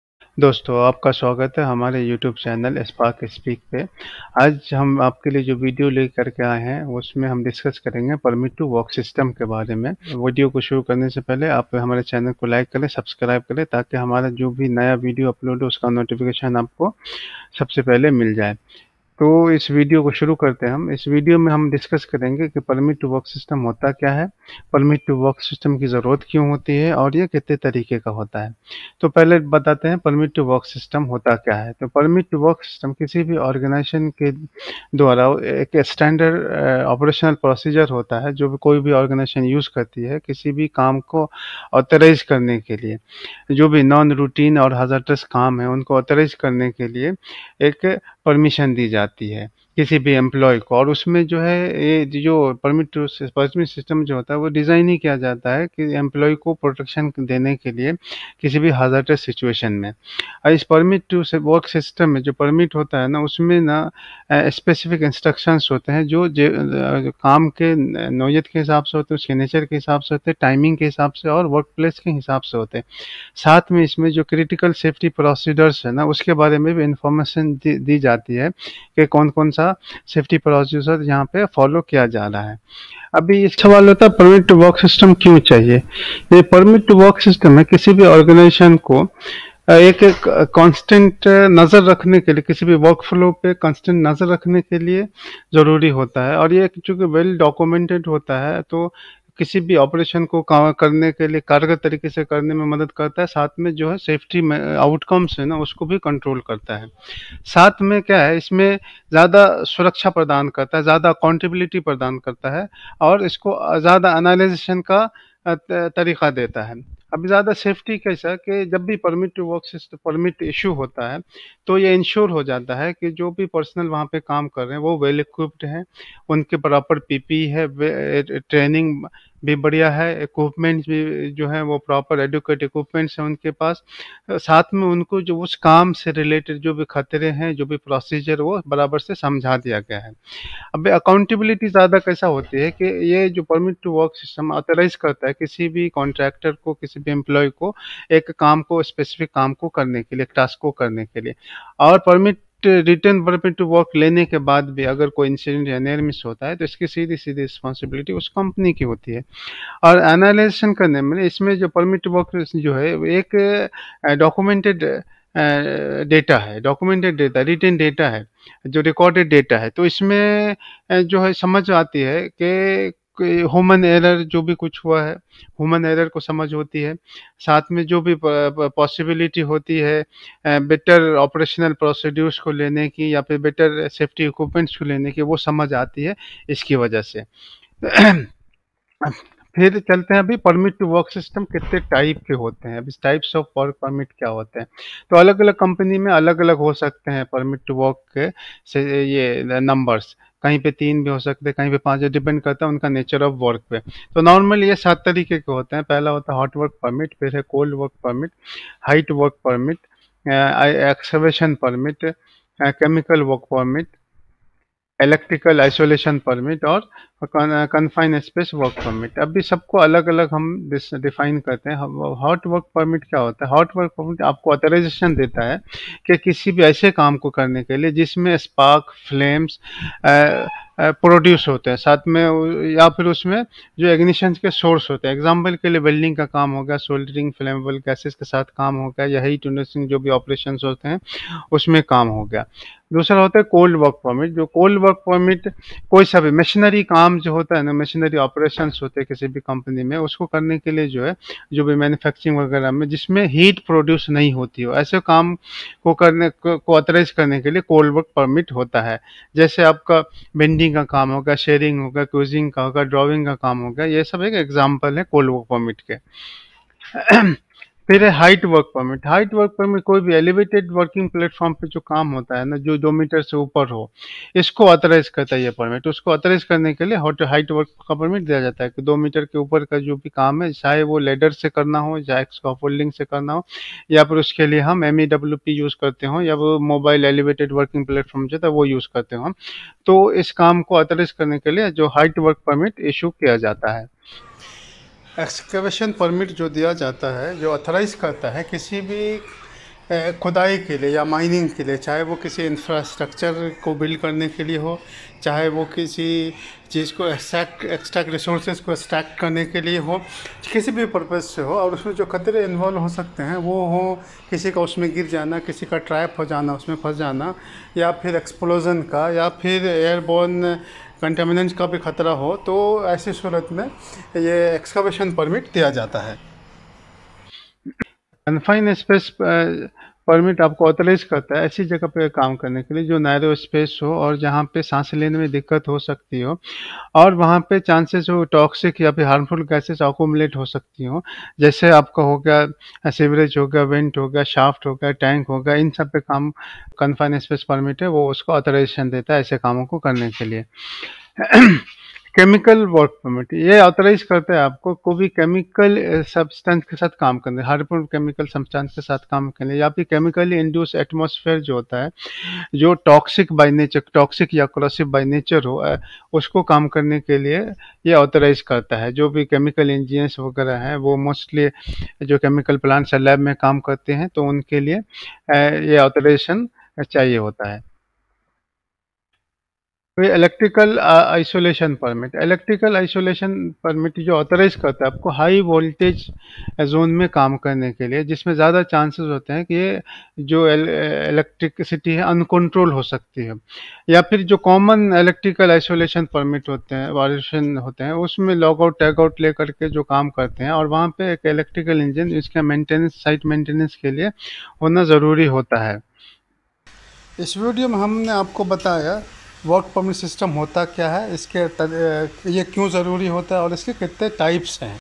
Permit to work system | Types of work permit | Spark Speak
In this Spark Speak video we are explaining about permit to work system,  their types and why ptw required.